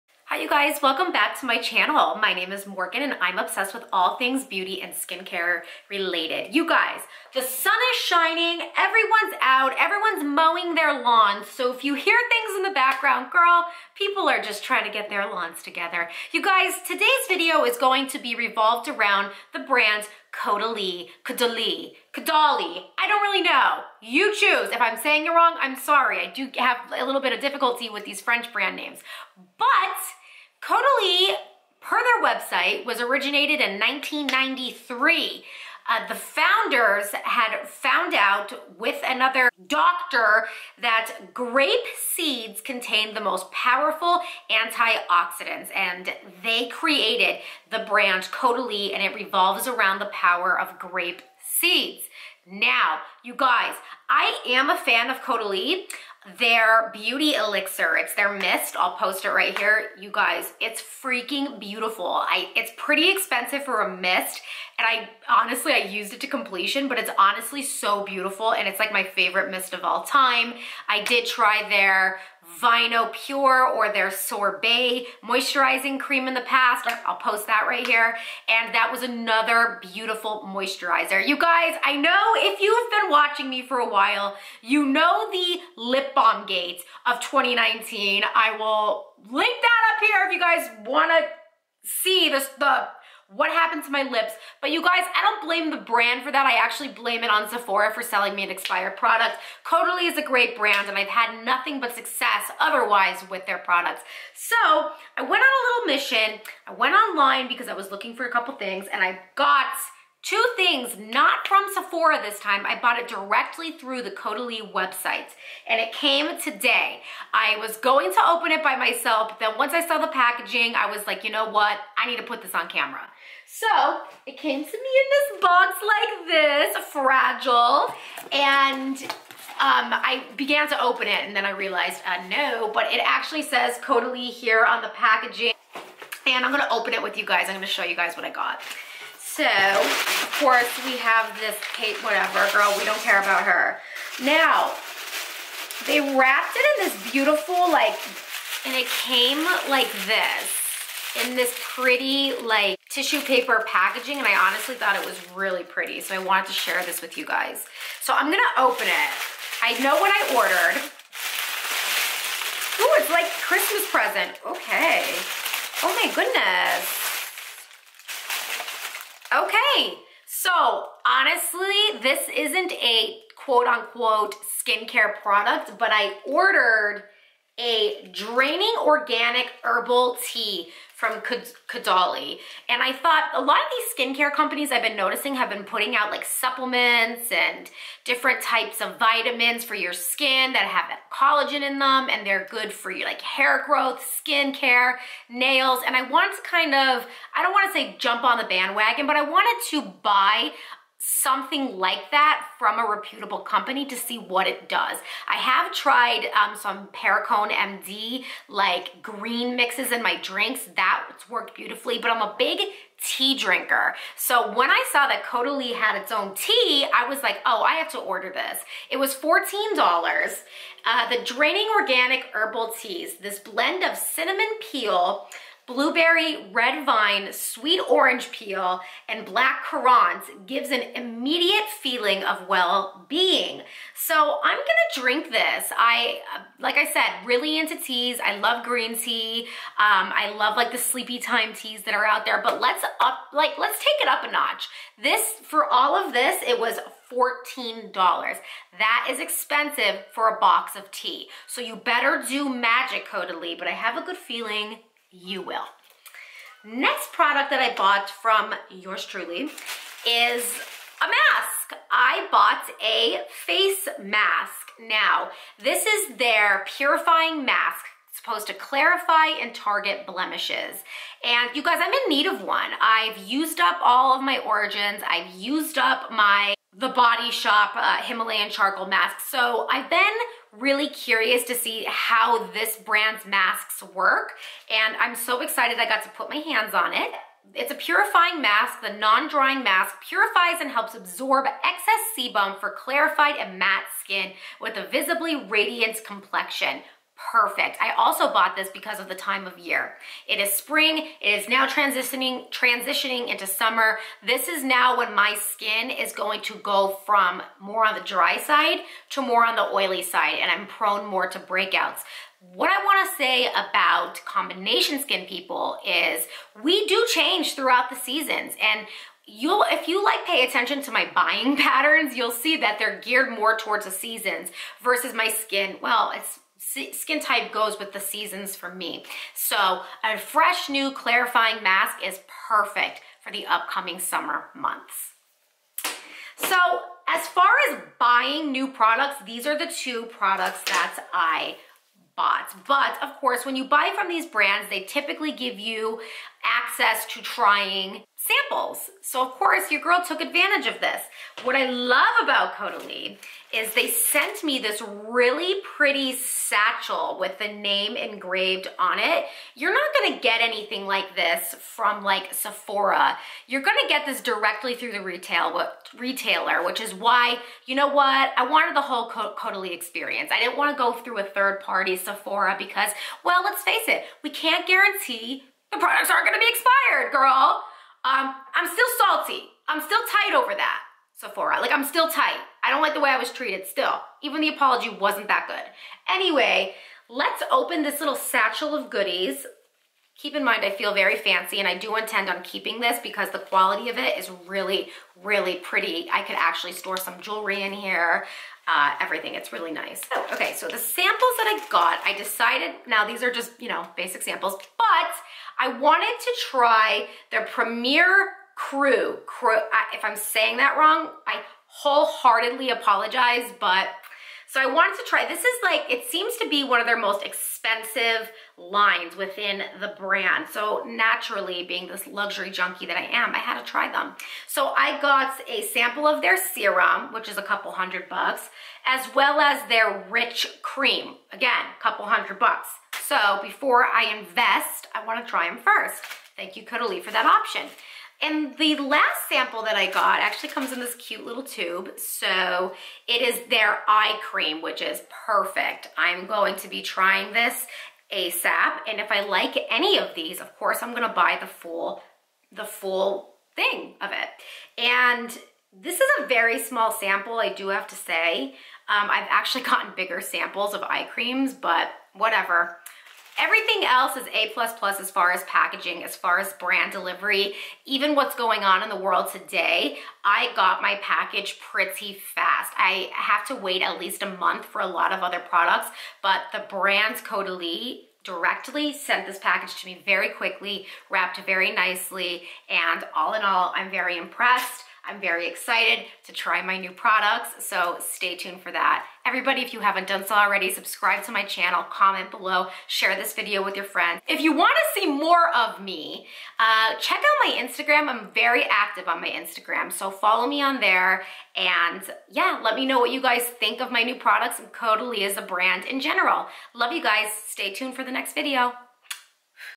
Caudalie Paris Unboxing NEW*!!
Welcome to my unboxing video ! I ordered from the luxury skincare brand, Caudalie Paris ! The packaging was so beautiful, I wanted to share this unboxing with you. Also, I like to share what new skincare products I have recently purchased. Skincare is a MAJOR passion of mine, so please join me on my luxury skincare journey ! :) Love you- Morgan

Products Mentioned:
Caudalie Paris- BEAUTY ELIXIR:
Caudalie Paris- VINOSOURCE MOISTURIZING SORBET:
Caudalie Paris- PURIFYING MASK:
Caudalie Paris- ORGANIC HERBAL TEA:
Caudalie Paris- PREMIER CRU THE RICH CREAM:
Caudalie Paris-PREMIER CRU THE SERUM: